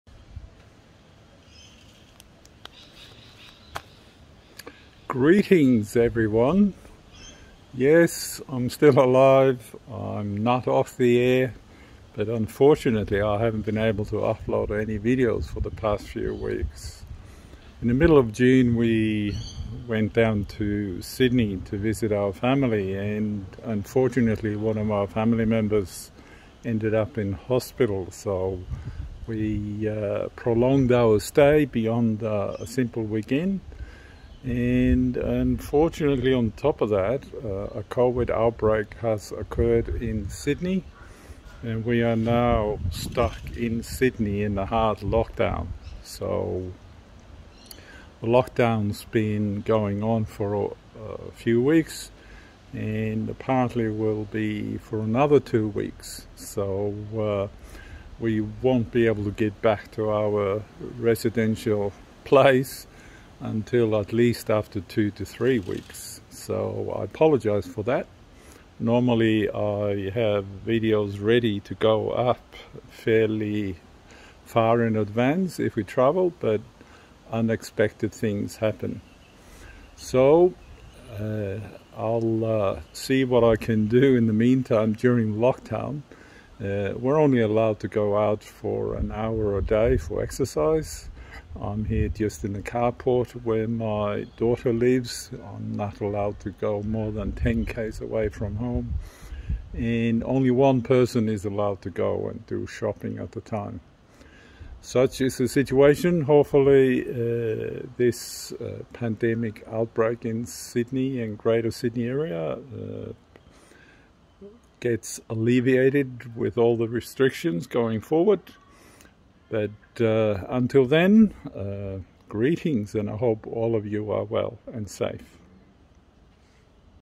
Update on the lack of videos
We are currently stuck in a hard lockdown in greater Sydney, due to a covid breakout. We have been unable to return back home where I usually edit and upload the travel videos.  We came to Sydney in mid June, intending to stay over a weekend, but had to extend our stay due to a family illness.. Then the breakout and lockdown occurred.  The lockdown is for at another two weeks at least, so as soon as I can I'll add more content..